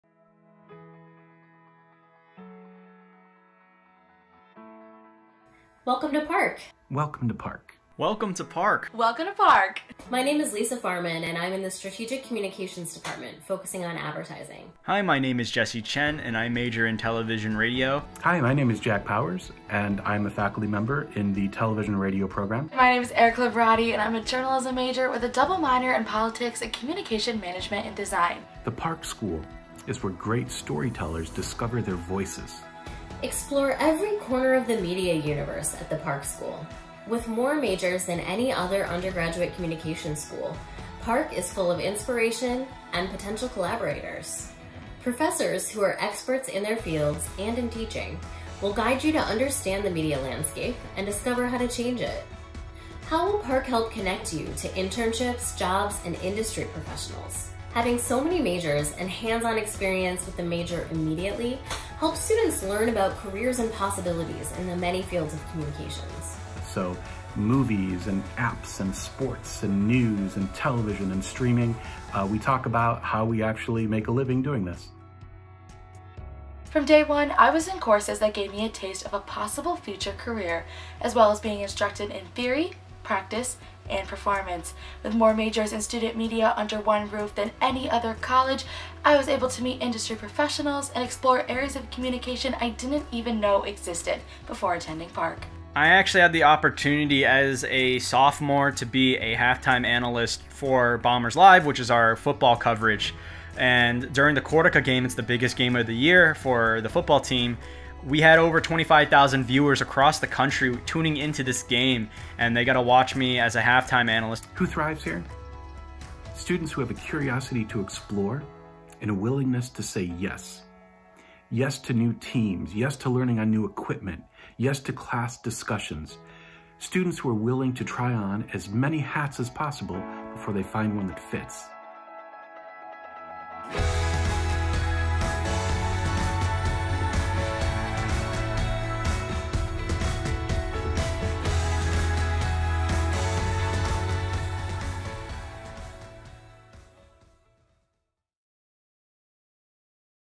Welcome to Park!
Experience the Park Difference! Hear from faculty and current students about what makes our school so special.

This is a student-produced video created by the co-curricular Park Productions. Even while quarantined, students and faculty maintain high spirits, collaborate remotely, and pull together available resources to bring to you their love of Park.